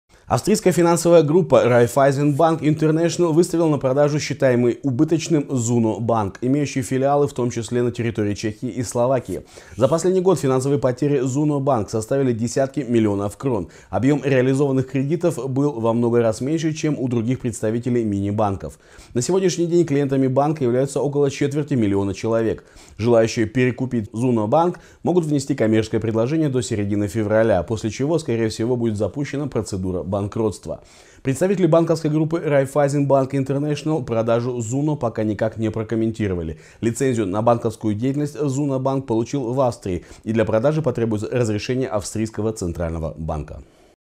Raiffeisen Bank International выставила на продажу убыточный Zuno Bank
Австрийская финансовая группа Raiffeisen Bank International, выставила на продажу, считаемый убыточным Zuno Bank, имеющий филиалы в том числе и на территории Чехии и Словакии. За последний год финансовые потери Zuno Bank составили десятки миллионов крон. Объем реализованных кредитов был во много раз меньше, чем у других представителей мини банков.

На сегодняшний день, клиентами банка является около четверти миллиона человек. Желающие перекупить Zuno Bank могут внести коммерческое предложение до середины февраля после чего, скорее всего, будет запущена процедура банкротства.

Представители банковской группы Raiffeisen Bank International продажу Zuno пока никак не прокомментировали. Лицензию на банковскую деятельность Zuno Bank получил в Австрии, и для продажи потребуется разрешение австрийского центрального банка.